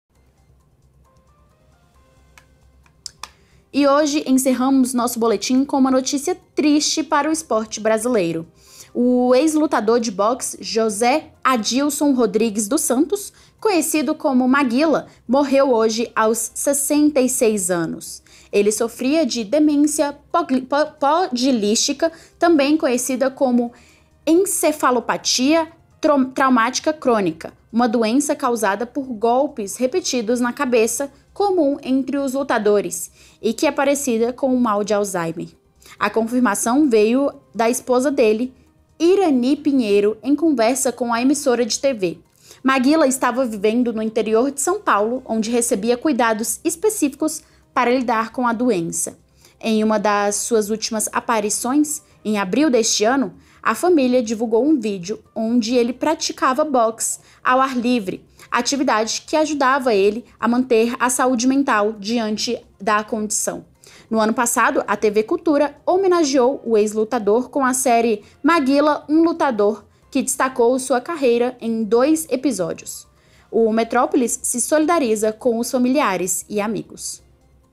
M*rre Maguila, ex-lutador de boxe, aos 66 anos
Morreu nesta quinta-feira (24/10), aos 66 anos, José Adilson Rodrigues dos Santos, conhecido como Maguila. O ex-lutador de boxe foi vítima de demência pugilística. Ele sofria de Encefalopatia Traumática Crônica (ETC); trata-se de uma doença incurável, similar ao mal de Alzheimer, causada por golpes frequentes na cabeça – motivo pelo qual atinge muitos lutadores.